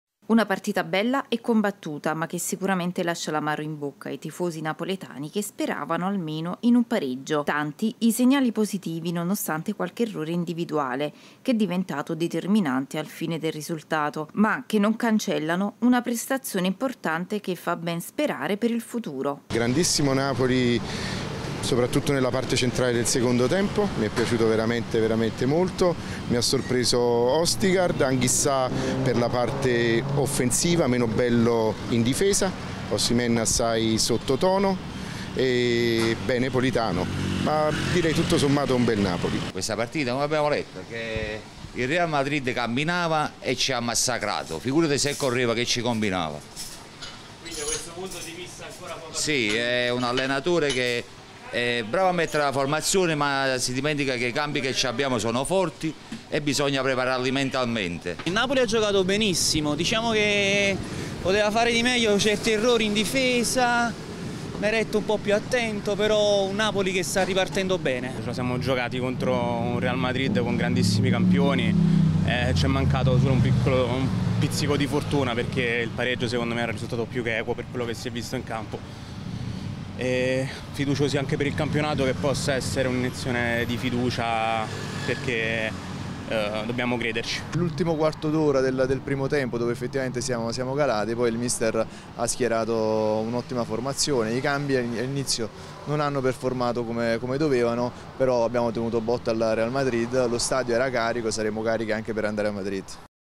Il Real Madrid sbanca il Maradona: il commento dei tifosi
Esordio casalingo sfortunato per il Napoli che perde con il Real Madrid per un'autorete: il commento dei tifosi azzurri.